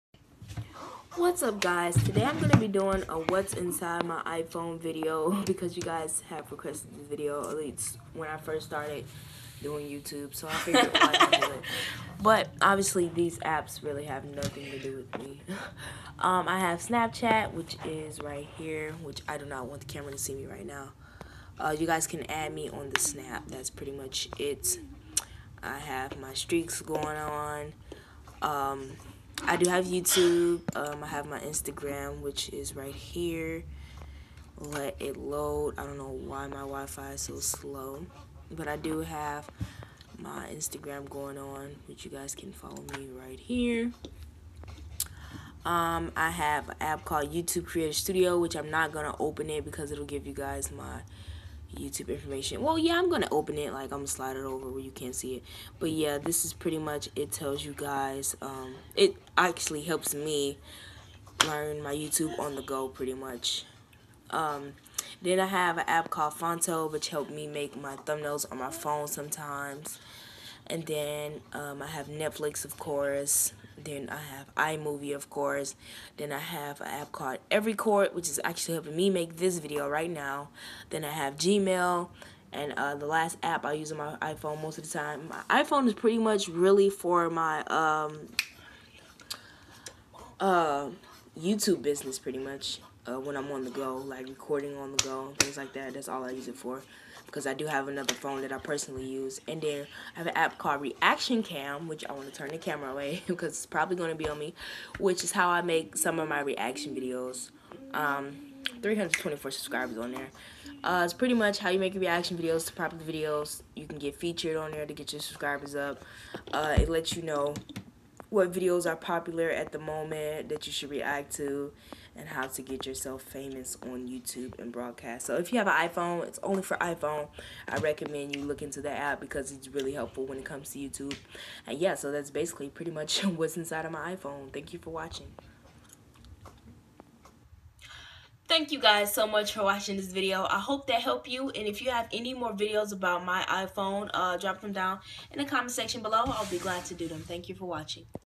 What's on my iPhone 6?
DONT BE STUPID !

THE IPHONE IS FOR YOUTUBE ONLY THE ANDROID IS FOR CALLS AND MESSAGES

IM SCREEN RECORDING AS WELL AS FILMING OFF OF MY IPHONE ! SO THATS WHY MY IPHONE ISNT IN THE VIDEO !!

THE ANDROID IN MY HAND IS WHAT I DO EVERYTHING OFF OF SO IN THIS VIDEO I WAS TEXTING MY MOM SO YES ITS AN ANDROID IN MY HAND !!

WHAT I AM SHOWING YOU IS THE INSIDE OF MY IPHONE THAT I USE FOR YOUTUBE ONLY ! THINK BEFORE YOU COMMENT